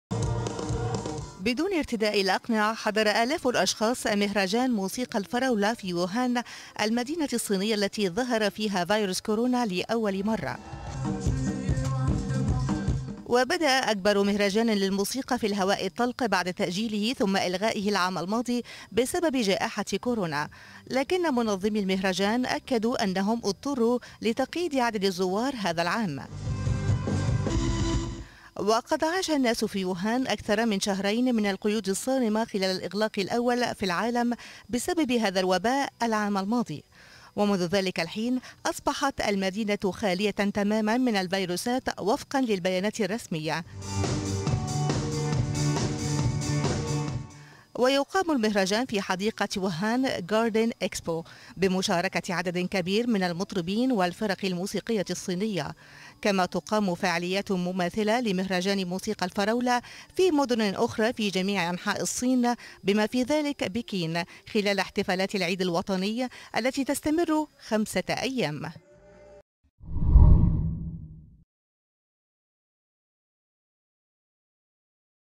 سكان ووهان يحتفلون لأول مرة منذ تفشي كورونا بمهرجان جماعي
للمرة الأولى منذ الغلق بسبب جائحة كورونا، يحتفل سكان مدينة ووهان الصينية بؤرة تفشي فيروس كورونا بمهرجان "موسيقى الفراولة".

 وتقام فعاليات المهرجان" في مدن صينية عدة، بما في ذلك بكين، خلال العيد الوطني الذي يستمر خمسة أيام في البلاد.

المزيد من التفاصيل في سياق التقرير.